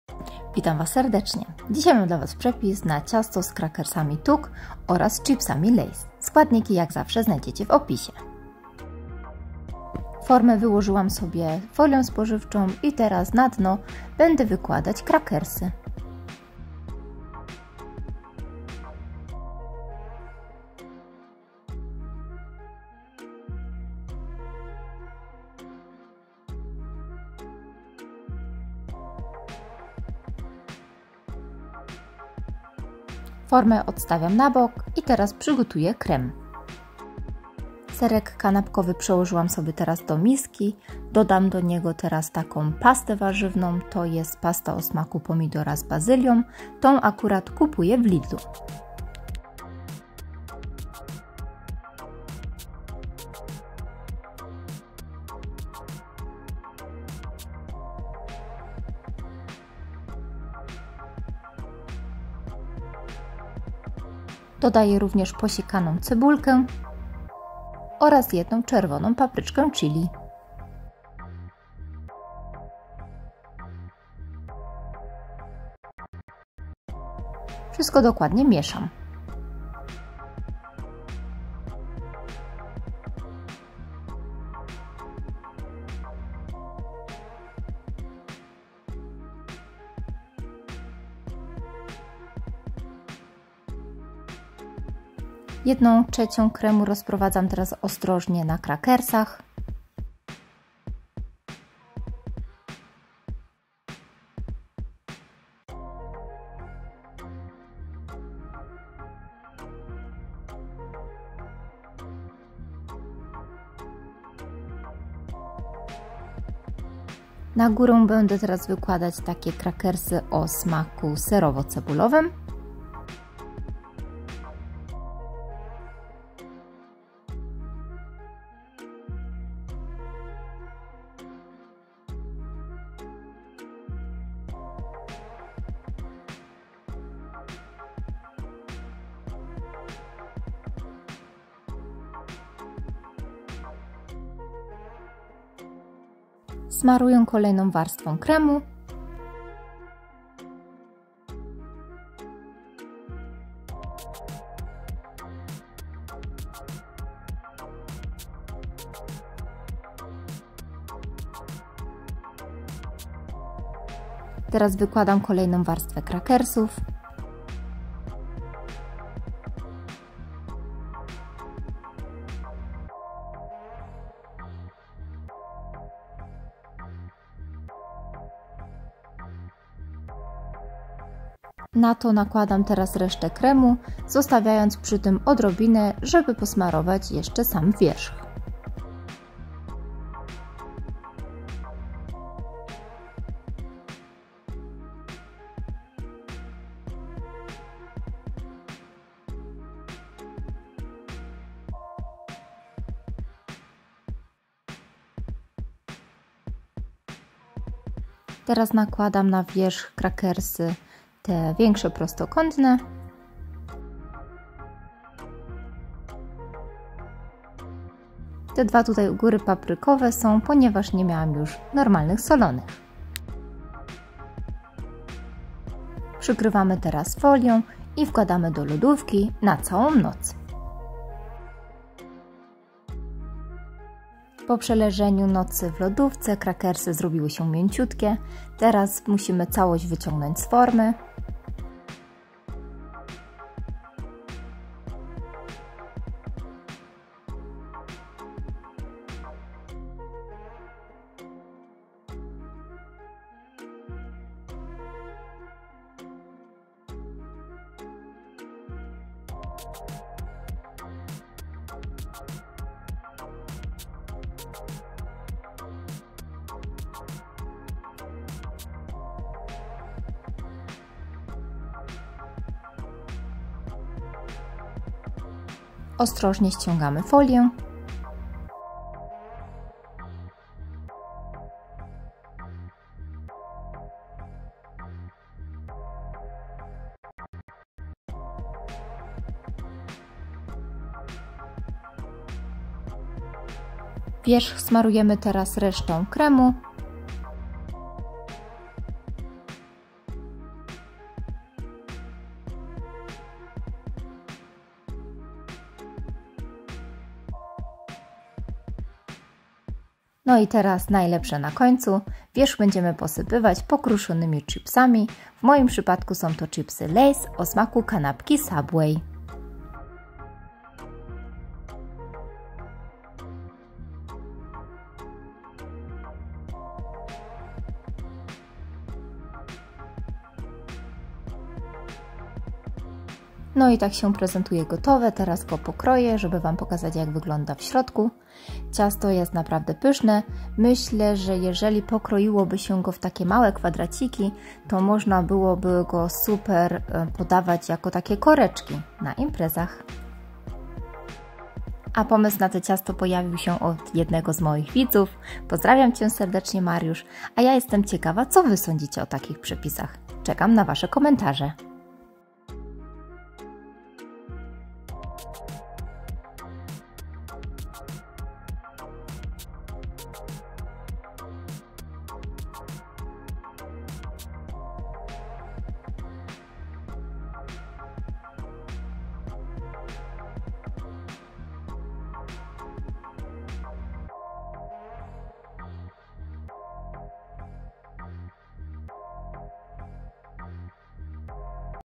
Ciasto LAY'S z krakersami TUC
Witam Was serdecznie.
Dzisiaj mam dla Was przepis na nietypowe ciasto.  Ciasto z krakersów TUC I chipsòw LAY'S.  idealne na imprezy.

Składniki: Forma 24x20cm

Ok.1,5 opakowania krakersów TUC solonych
400g serka kanapkowego śmietankowego
słoiczek pasty warzywnej o smaku pomidora z bazylią ( 180g)
3 cebulki dymki
1 papryczka czerwona chilli
1 opakowanie krakersów TUC baked bites o smaku serowo-cebulowym ( 110g)
chipsy LAY'S ok.20g